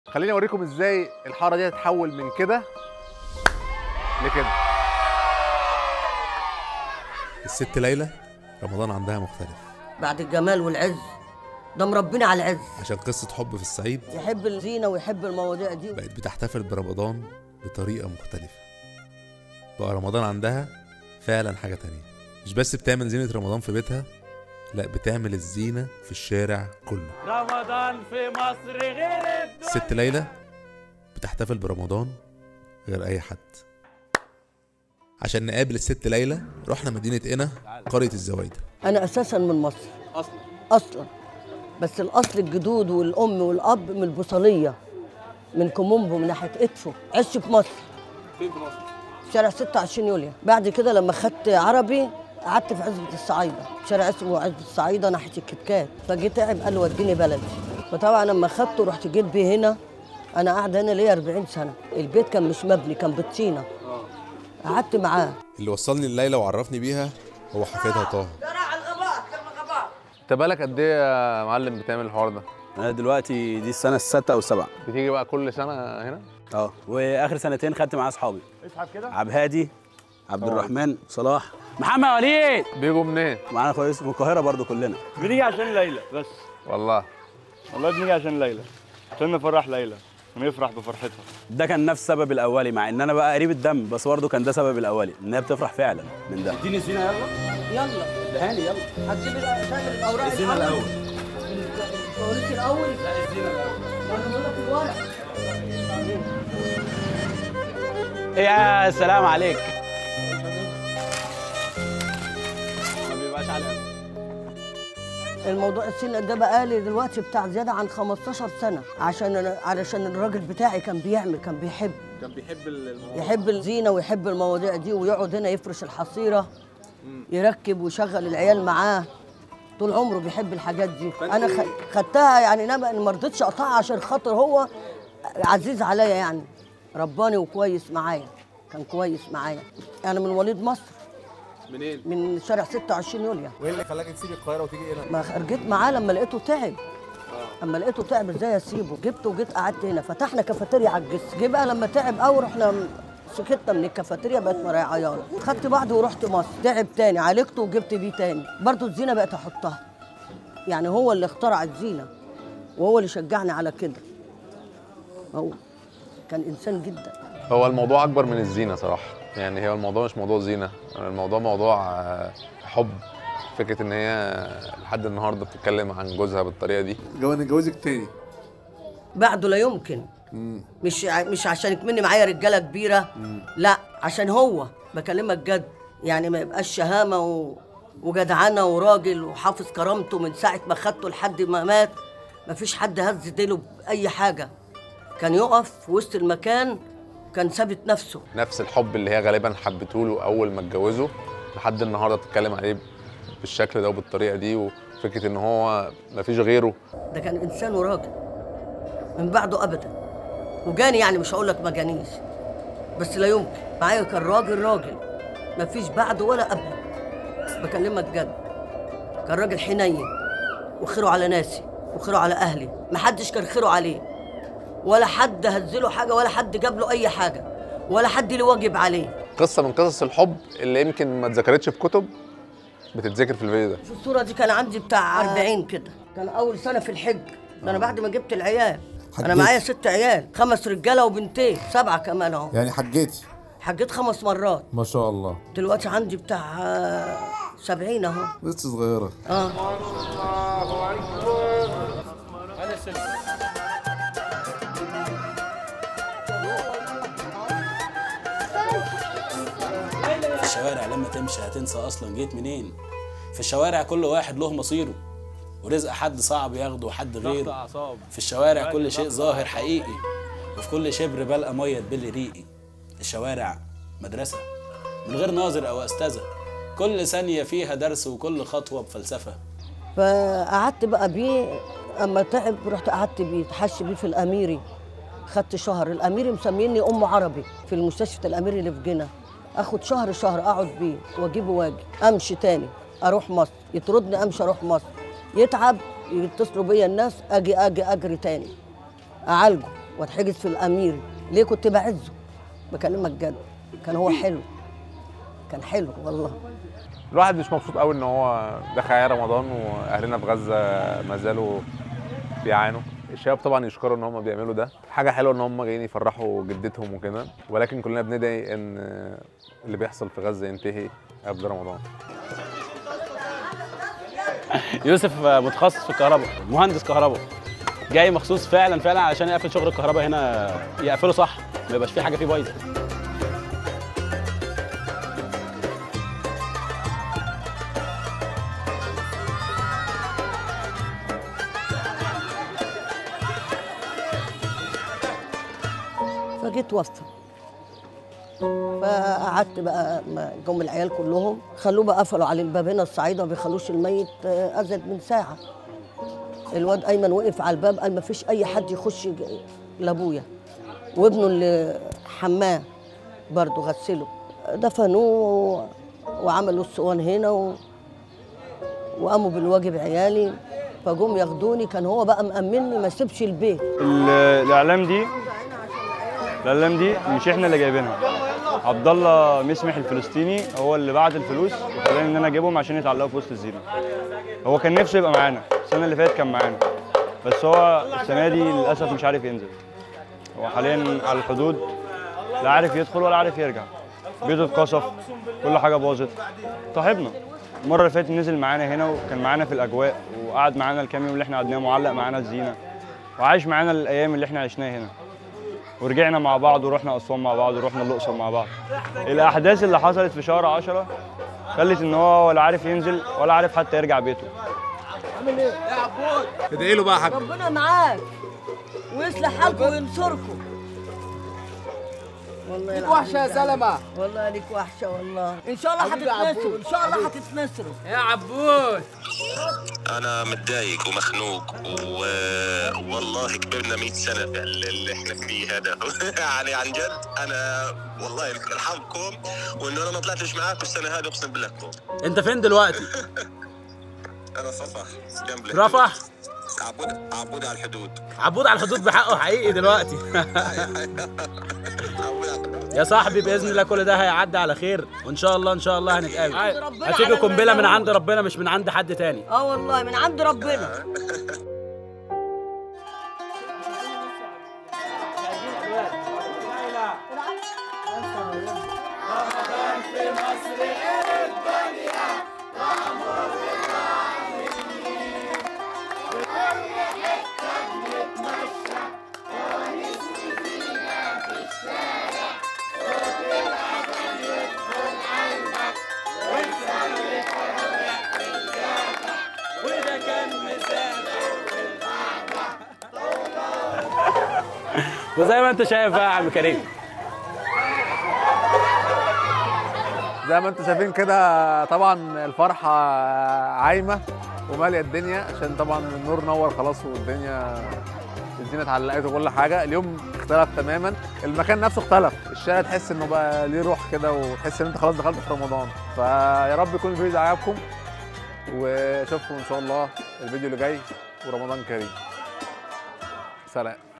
ست في صعيد مصر بتحتفل برمضان غير اى حد
رمضان في مصر وبالتحديد عند الست ليلى مختلف جدا
Ramdan music by @hamzanamira

English subtitle by Heba Mahmoud